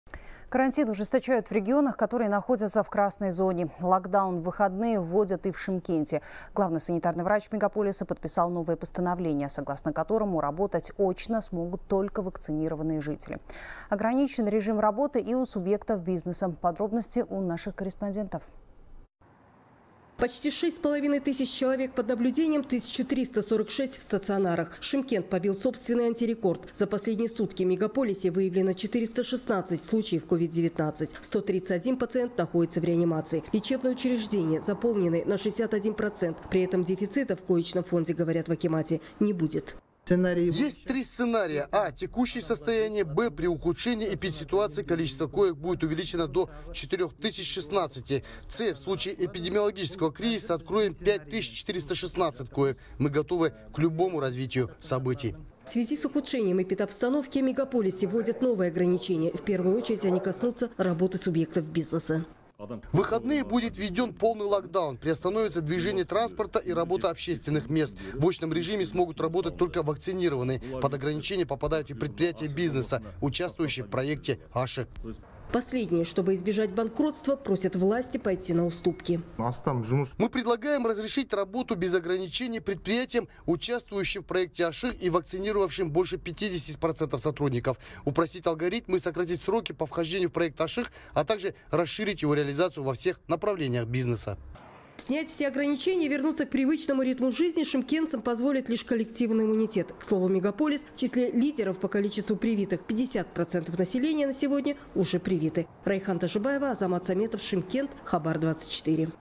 Локдаун в выходные вводят в Шымкенте
Карантин ужесточают в регионах, которые находятся в «красной зоне». Локдаун в выходные вводят и в Шымкенте. Главный санитарный врач мегаполиса подписал новое постановление, согласно которому работать очно смогут только вакцинированные жители, передает корреспондент «Хабар 24».

Ограничен режим работы и у субъектов бизнеса.